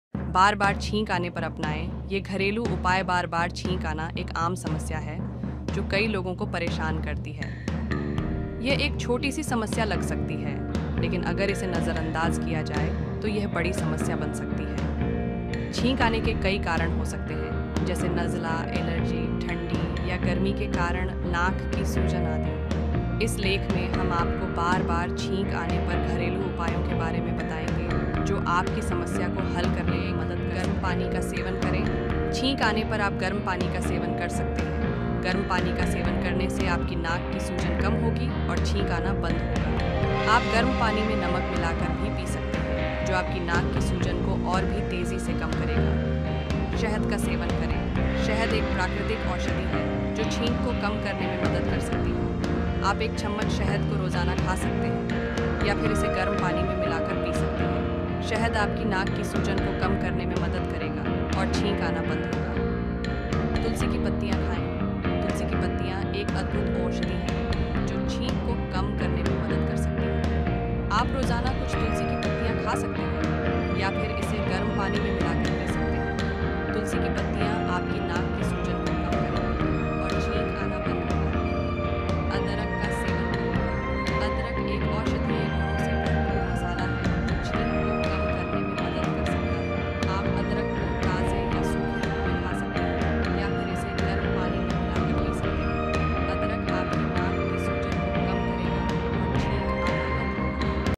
छींकने के अंधेरे रहस्यों को उजागर करना
छींक एक सामान्य श्वासन विकार है जो हमारे शरीर के नाक के मुख से आवाज़ के रूप में निकलता है। यह एक स्वाभाविक क्रिया है जो हमारे शरीर को बाहरी कणों, धूल और अन्य रोगाणुओं से सुरक्षा प्रदान करती है। हालांकि, कभी-कभी यह छींक बहुत अधिक हो जाती है और हमारे दिनचर्या को प्रभावित करती है। इस लेख में, हम आपको बार-बार छींक आने पर घरेलू उपायों के बारे में बताएंगे जो आपको इस समस्या से निजात दिलाने में मदद करेंगे।